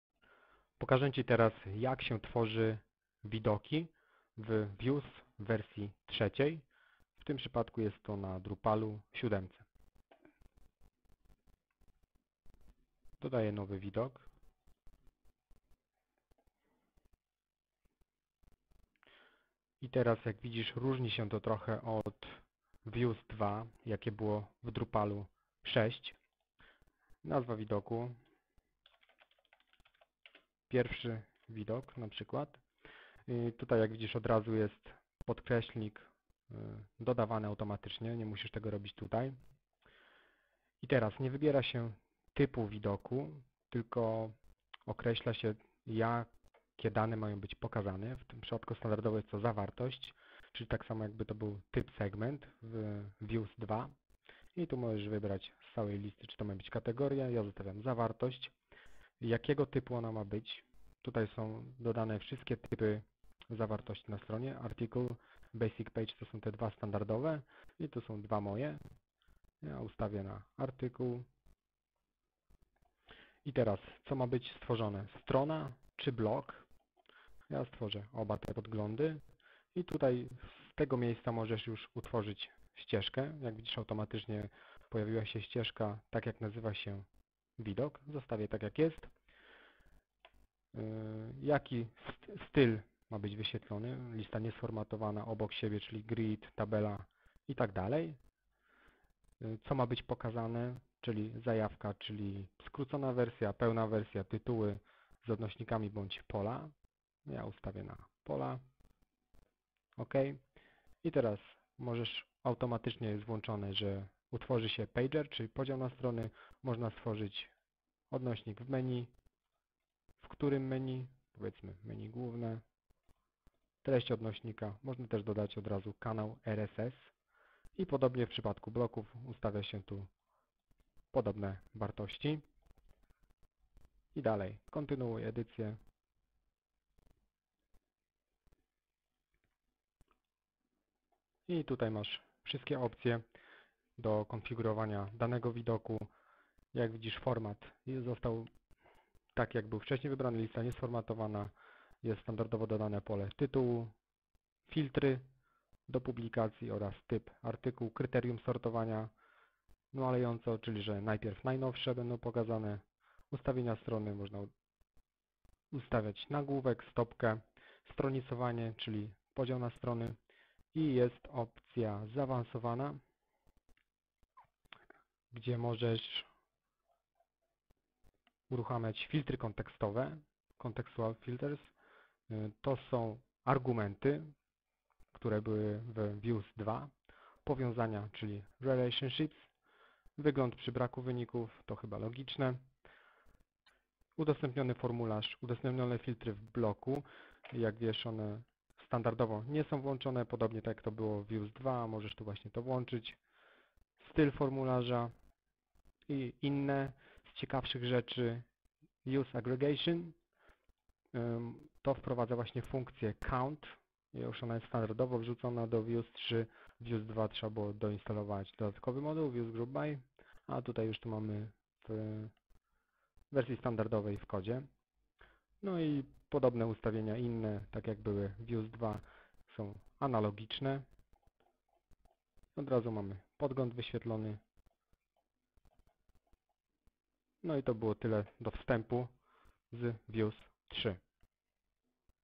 Wstep do Views 3 na Drupalu 7
Pierwsze spojrzenie na Views 3. Jak tworzyć nowe widoki. Więcej na